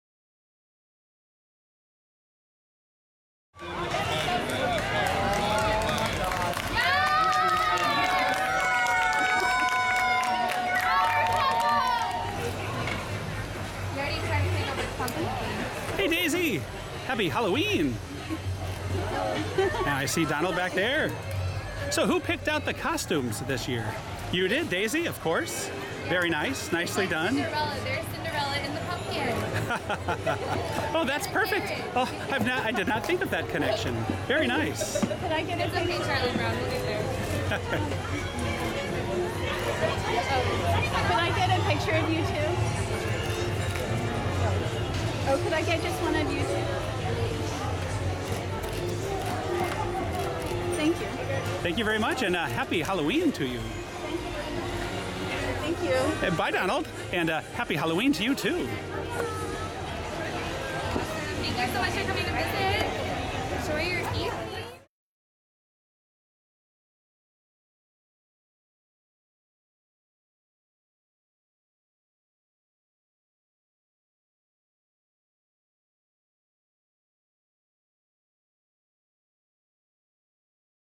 Pumpkin Donald & Princess Daisy Duck Meet & Greet at Mickey's Not So Scary Halloween Party 2019
We met Donald and Daisy in their Halloween costumes at Mickey's Not-So-Scary Halloween Party - Daisy was dressed as a princess and Donald as a pumpkin. They can be found at Storybook Circus in the Magic Kingdom during the party.